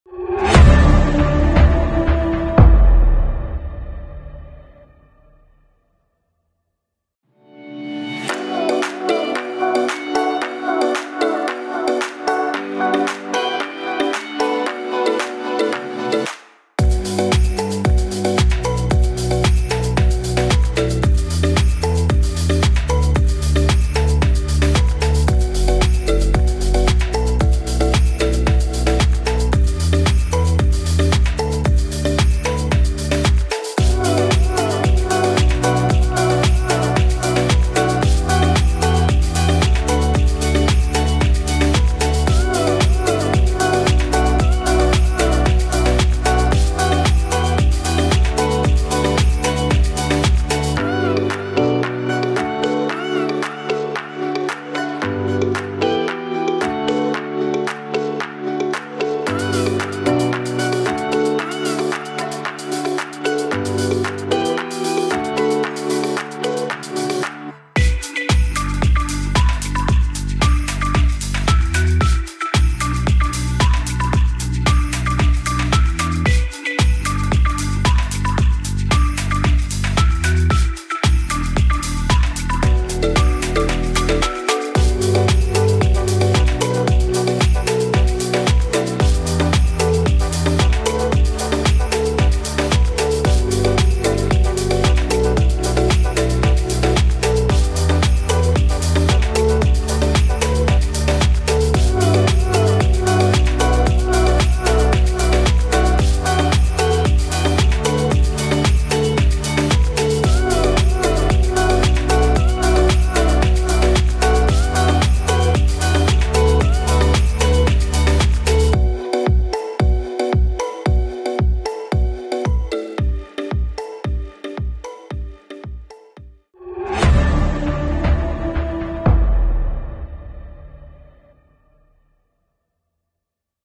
Racecourse Park, Northampton, UK- short footage with music(canon eos 6D mk ii + EF 50mm f/1.8)
A moment of fun through Racecourse Park, in Northampton, UK.
A short video footage with music using canon eos 6D mk ii and EF 50mm f/1.8 prime lens.